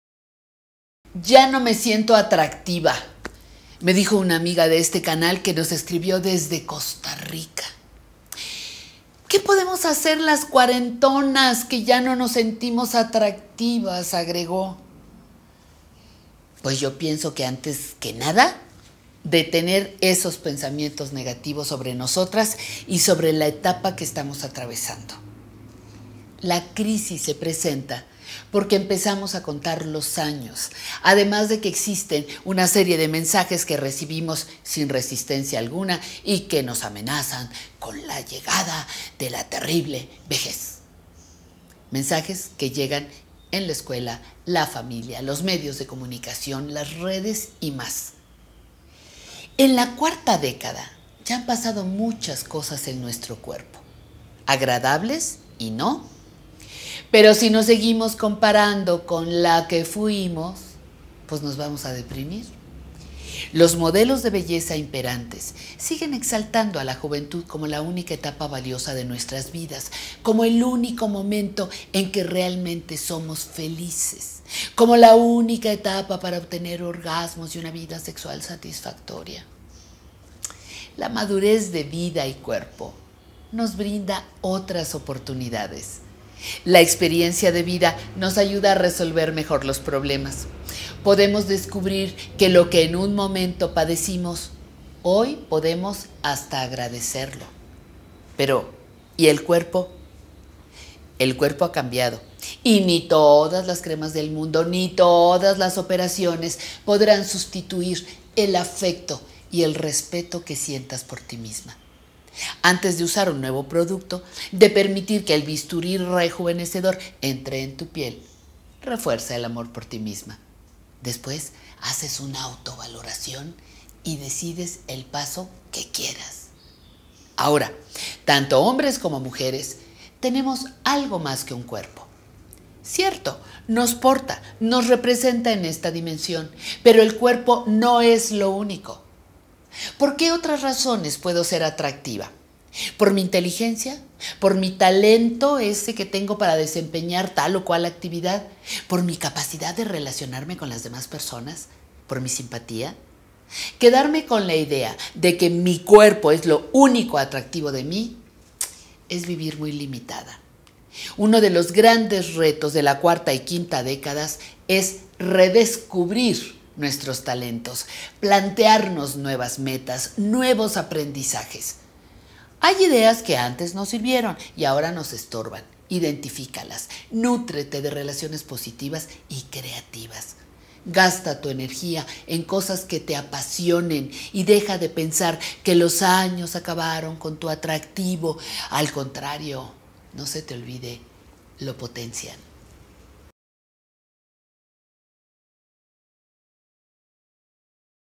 Ya no me siento atractiva | Vejez creativa con Patricia Kelly
Algunas personas suelen pensar que su cuerpo se ha devaluado con la vejez y se muestran incapaces de ver otras cualidades para mantener su valía en este sistema.  VEJEZ CREATIVA ofrece algunas ideas para que descubras y desarrolles tus nuevos talentos.